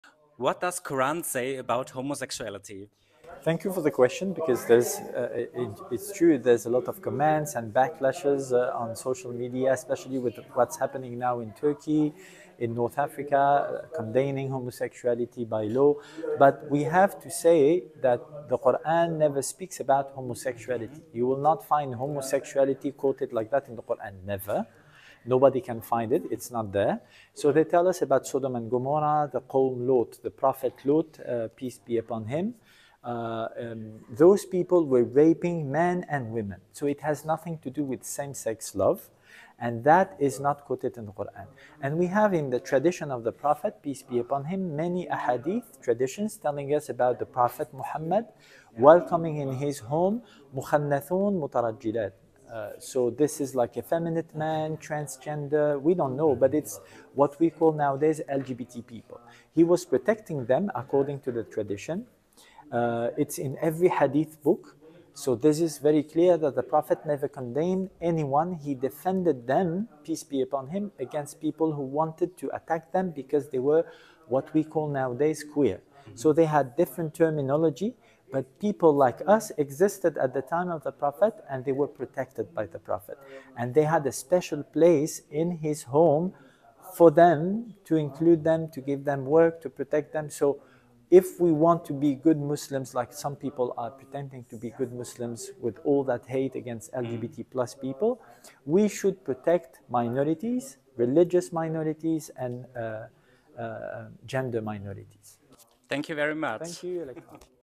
What does Qur'an say about Homosexuality?
Dr. Ludovic-Mohamed Zahed hat uns zur Gebetsraumeröffnung in Frankfurt besucht und ein Interview für Electra Pain gegeben. Danke an euch beide!

Dr. Ludovic-Mohamed Zahed visited us for the opening of our prayer room in Frankfurt and was interviewed by Electra Pain. Thank you both so much for this video!